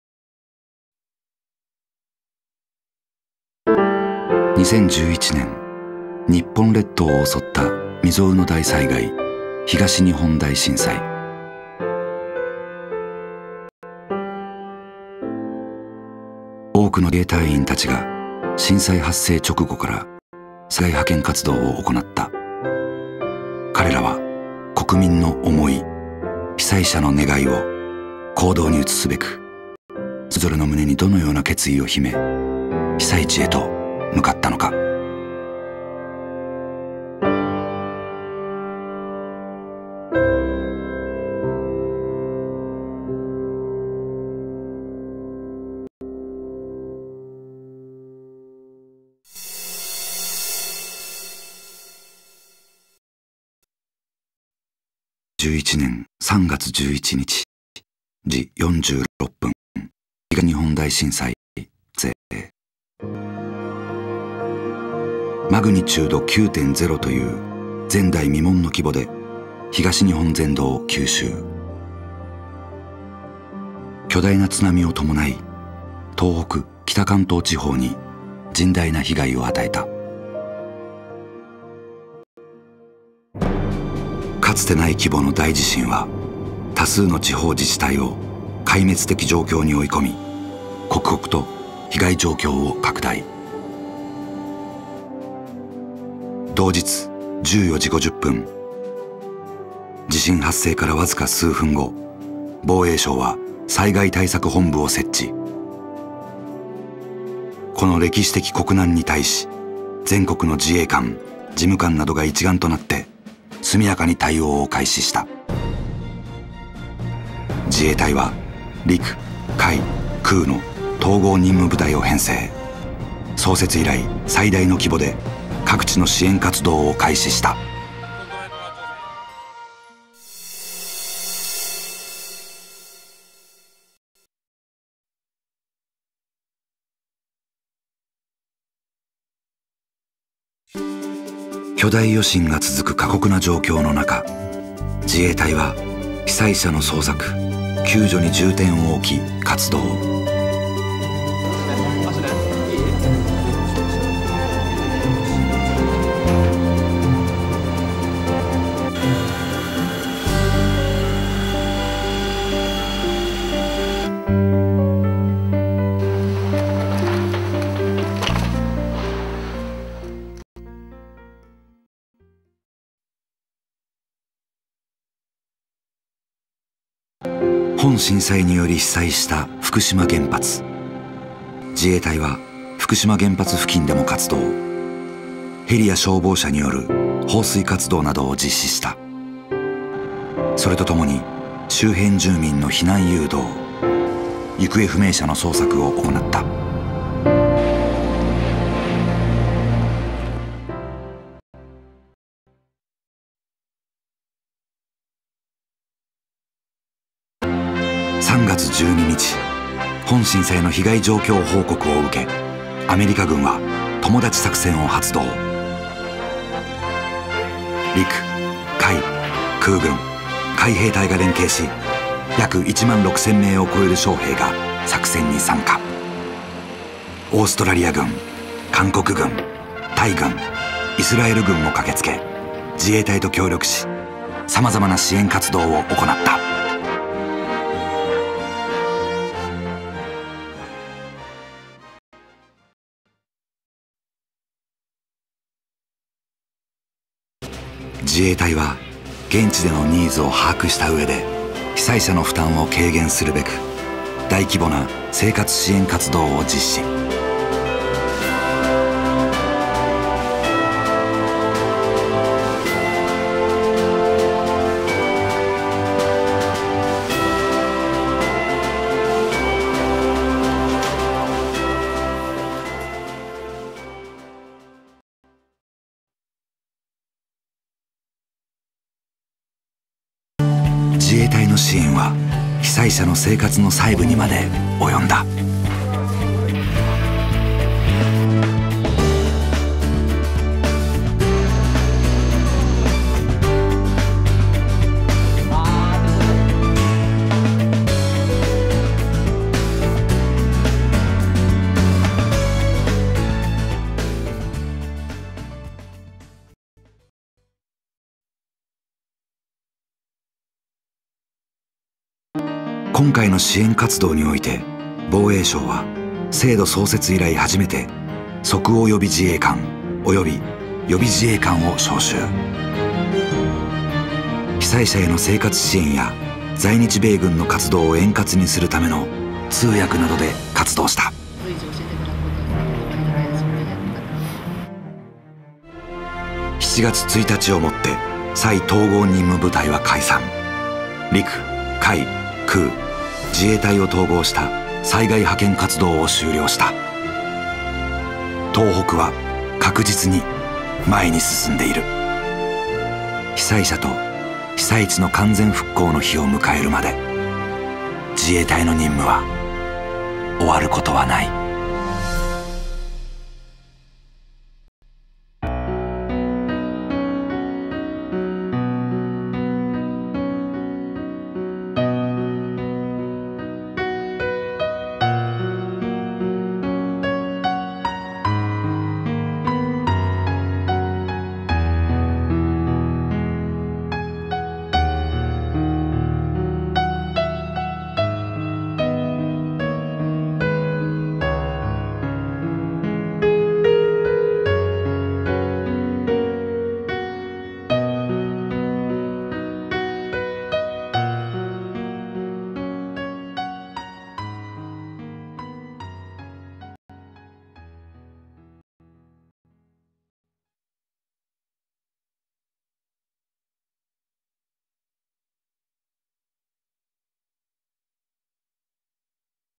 【ダイジェスト版】東日本大震災 災害派遣活動記録映像
東日本大震災における防衛省・自衛隊の災害派遣活動の記録映像です。